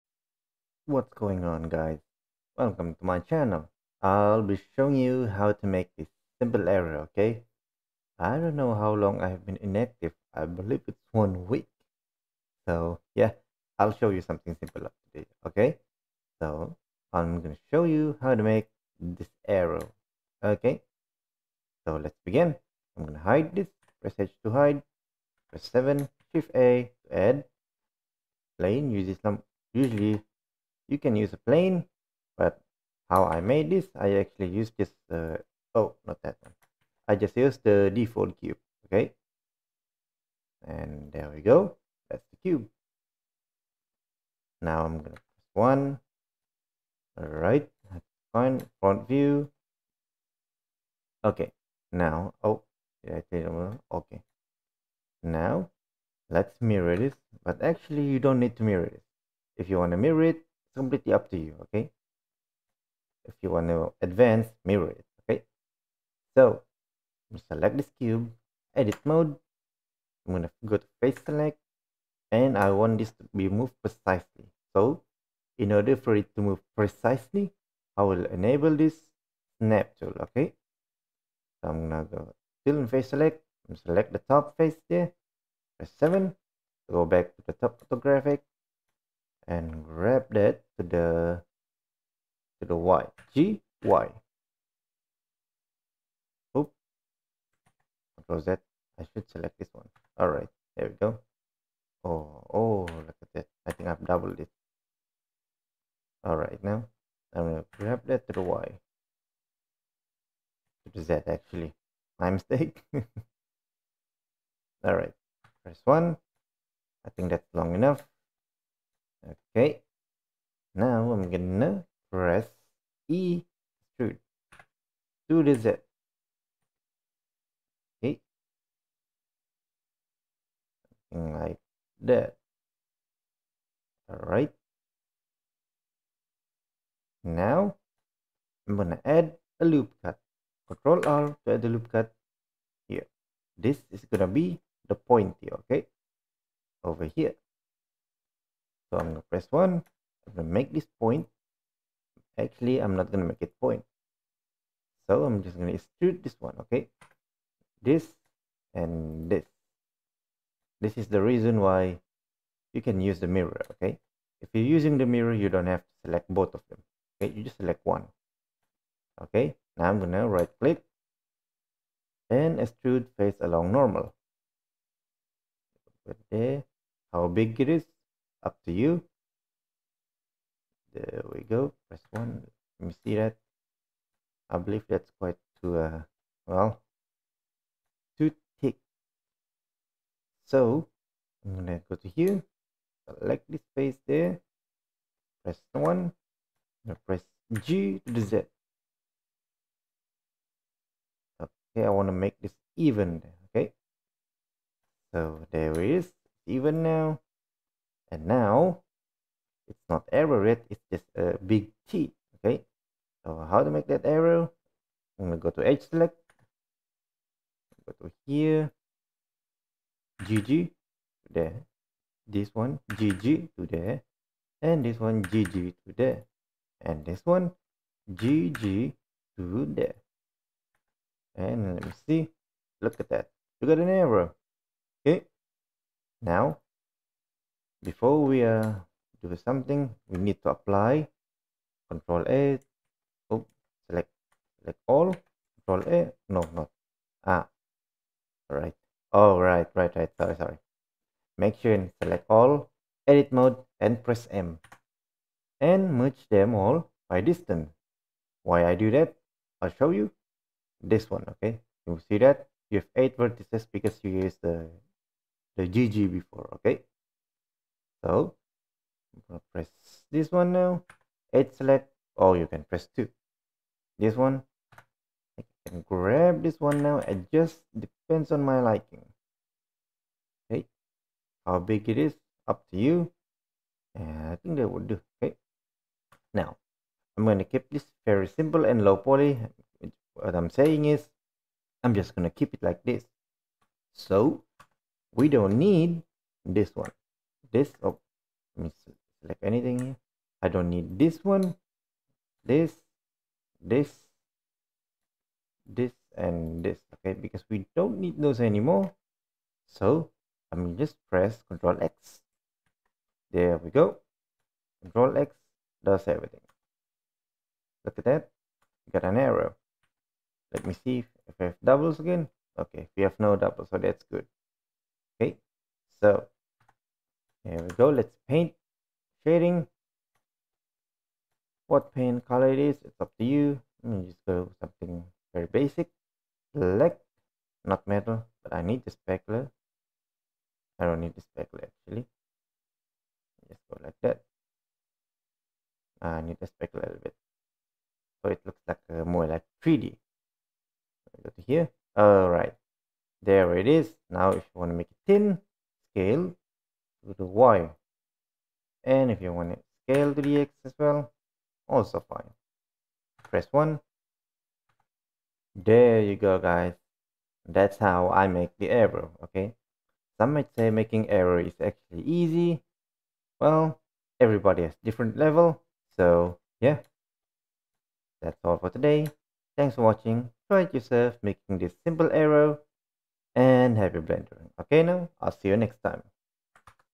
Blender Tutorial | Arrow shape
Whats up guys! Its been a week i've not making any tutorial. Here a simple and easy to learn in Blender, making an arrow. Note that there are other ways making this as well.

Audio kinda bit different now because i'm using a headphone mic now.